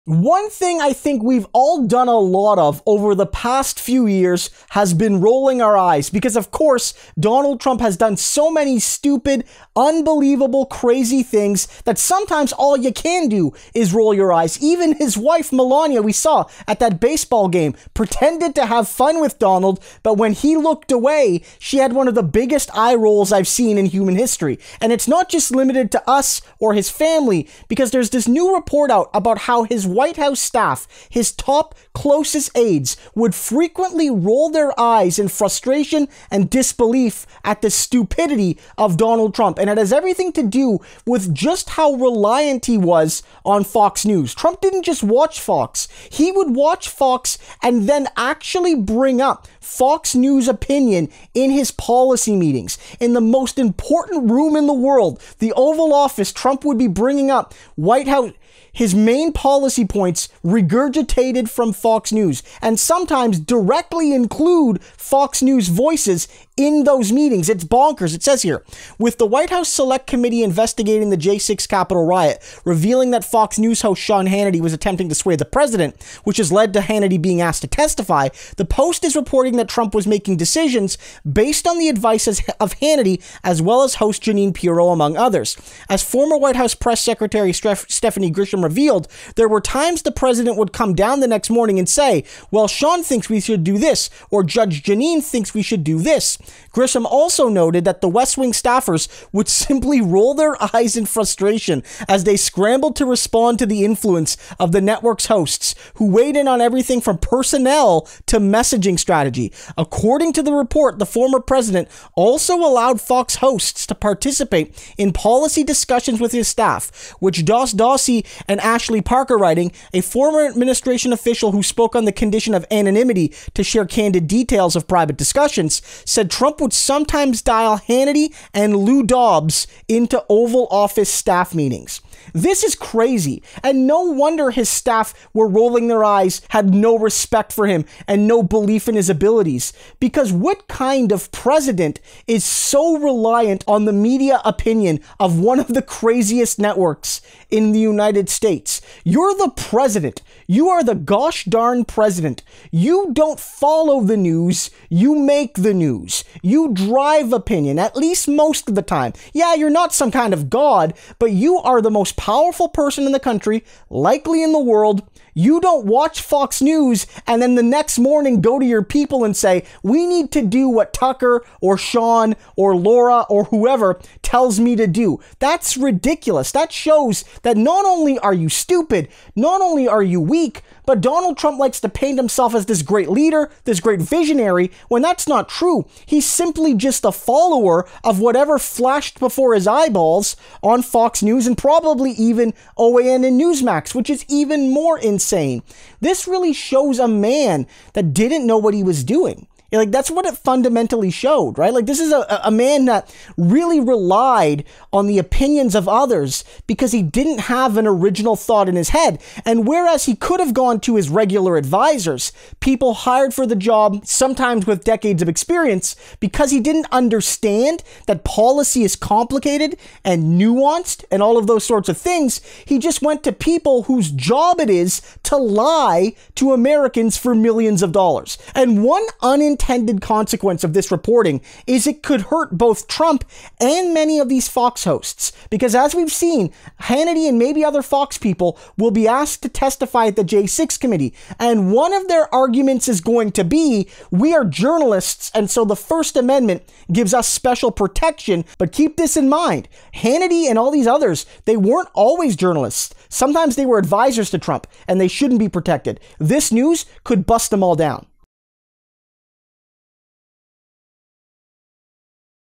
Trump Aides Spill HUMILIATING Secret about him
Donald Trump would often seek the advice of people he liked on the news, even though this caused his core advisers to lose total respect for him and roll their eyes in frustration